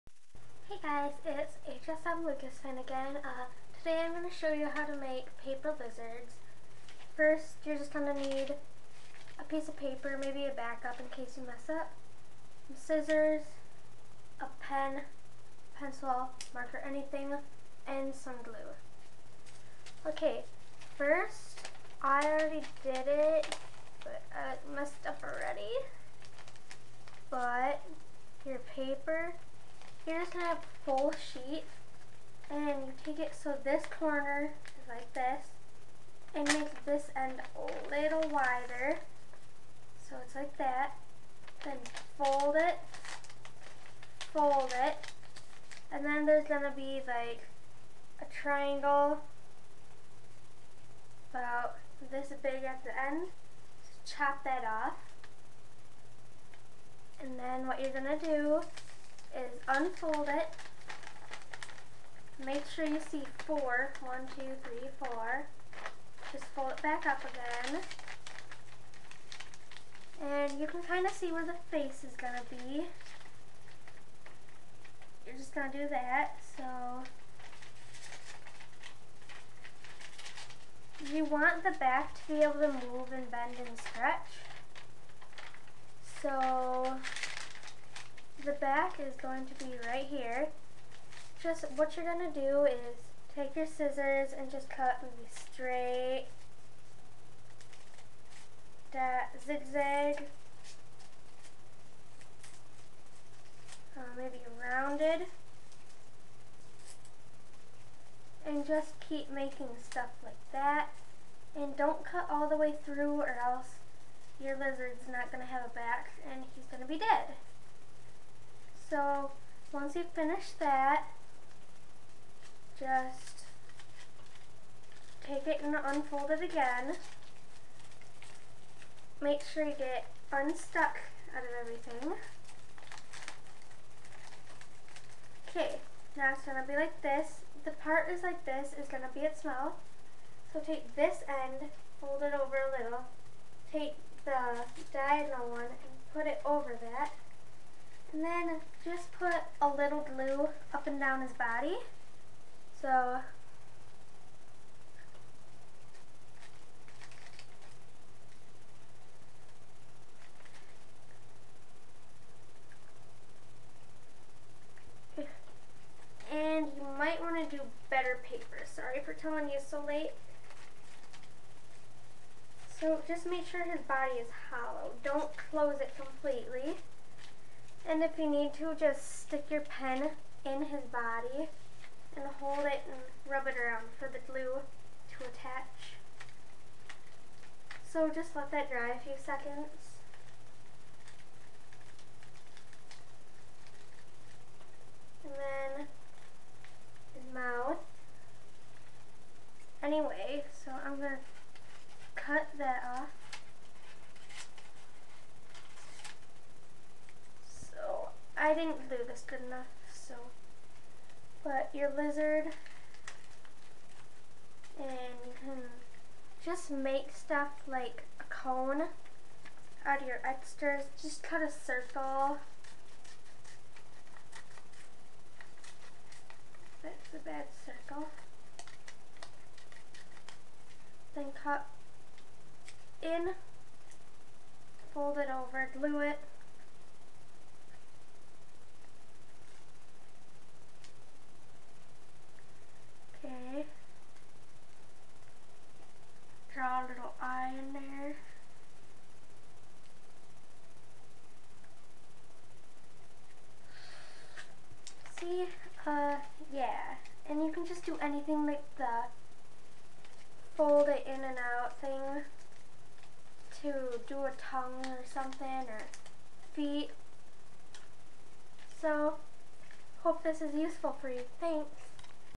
paper crafts.AVI
im making a paper lizzard that i learned in art class at school. they look real better with colored construction paper i hope you like them